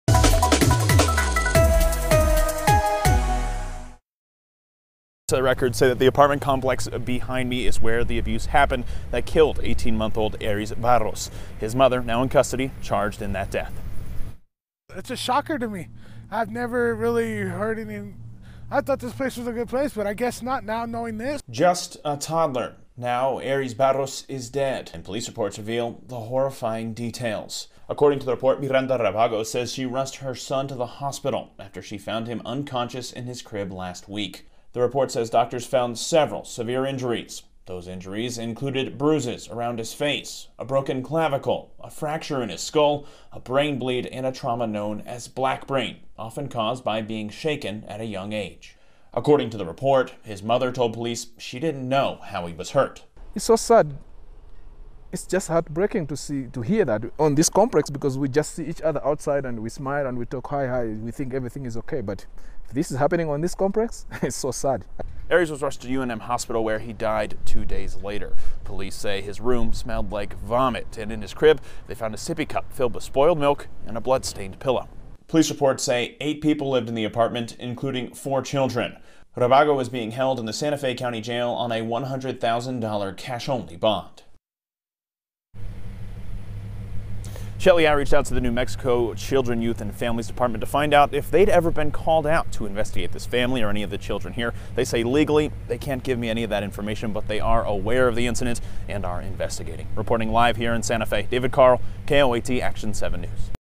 VIDEO: Mother charged with child abuse after toddler dies
Police say the toddler’s room smelled like vomit. They found a sippy cup in his crib filled with spoiled milk and a bloodstained pillow.